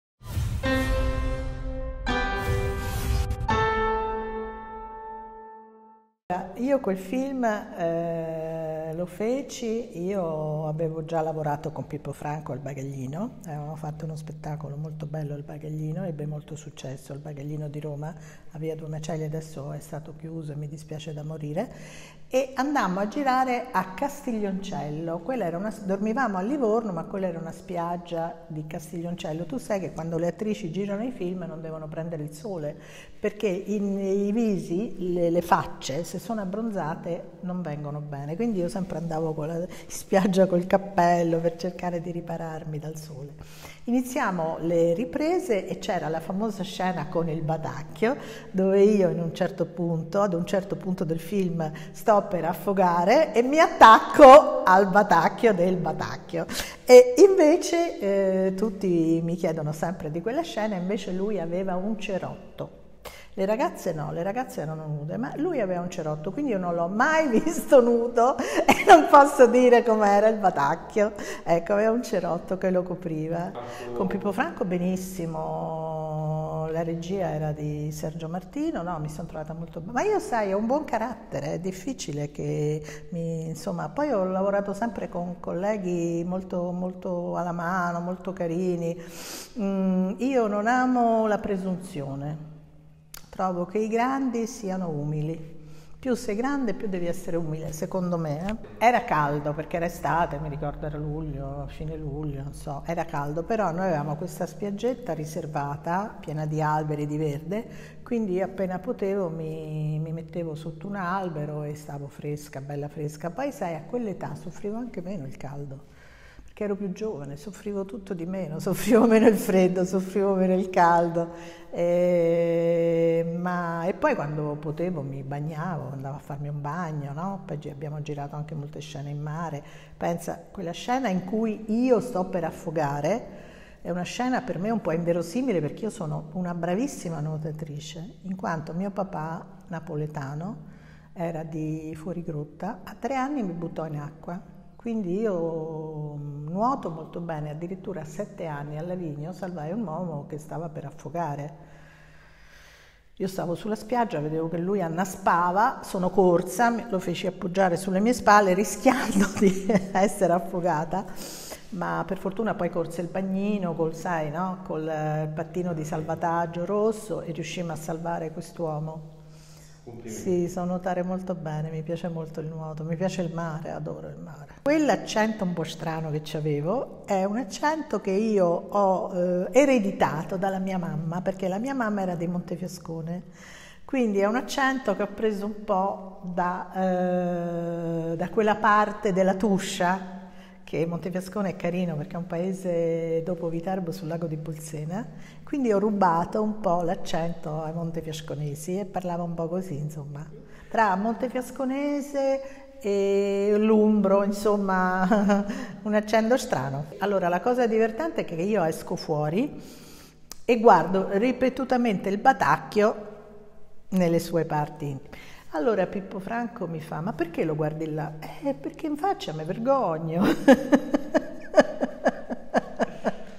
"Ricchi, ricchissimi...": Adriana Russo svela i retroscena della famosa scena con Batacchio
Ricchi ricchissimi, delitto sull’autostrada, brutti sporchi e cattivi, e tantissimi altri. Ecco a voi la nostra intervista ad Adriana Russo, realizzata nello splendido Palazzo Rospigliosi di Zagarolo.